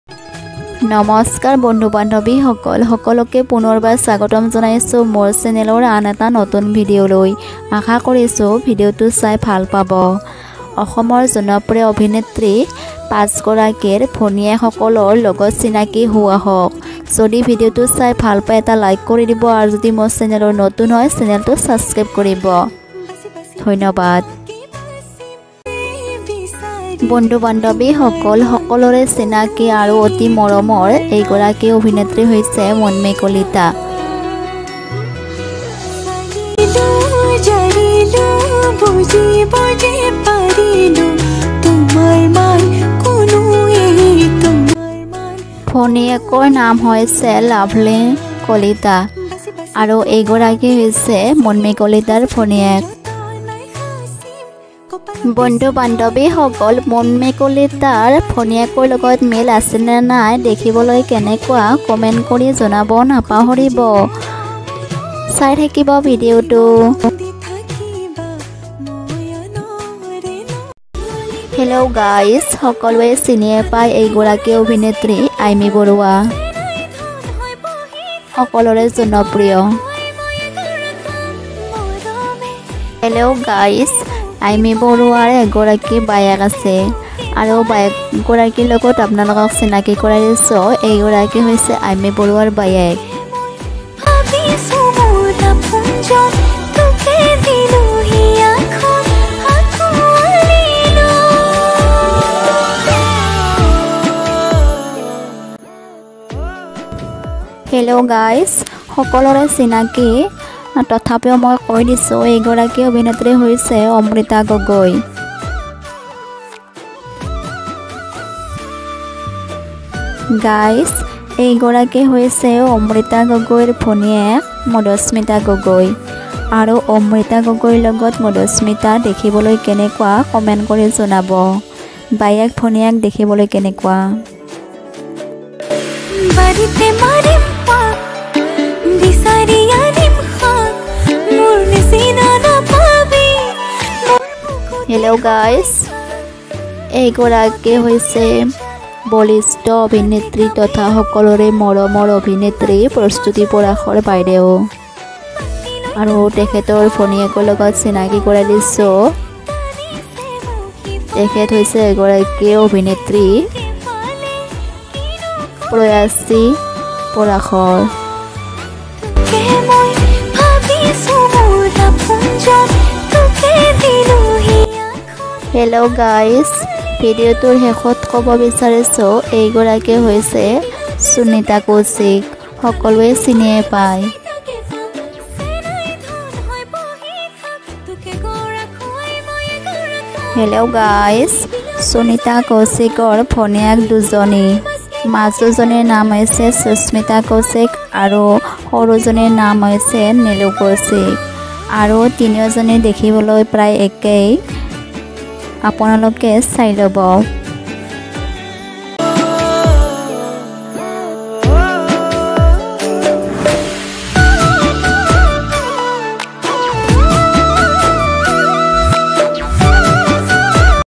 জনপ্ৰিয় ৫ গৰাকী অভিনেত্রীৰ বায়েক ভনীয়েক সকলৰ লগত চিনাকী হওঁ আহক। মৰম দিব 🙏
১/ অমৃতা গগৈ- মধুস্মিতা গগৈ
২/ প্ৰস্তুতি পৰাশৰ - প্ৰয়াশী পৰাশৰ
৩/ মুনমি কলিতা - লাভলি কলিতা
৪/ চুনিত‍া কৌশিক - সুস্মিতা -
৫/ আইমী বৰুৱা -